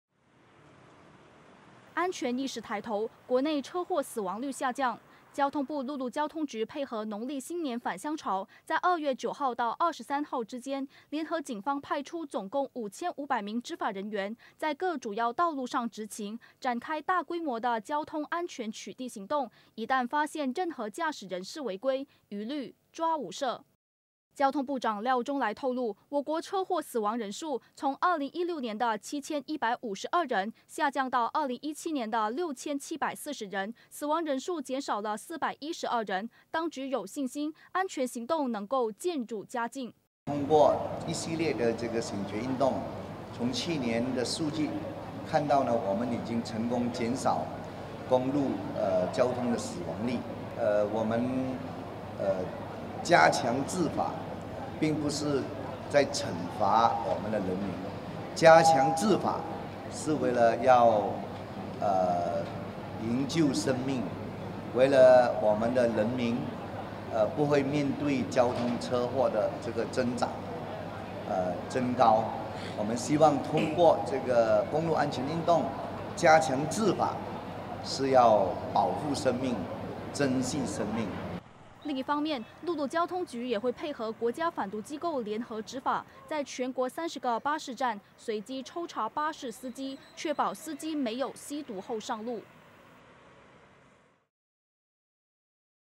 10/02: 安全行动奏效 国内车祸死亡率减少
农历新年安全行动开跑，全国5千名执法人员出动捉交通违例者。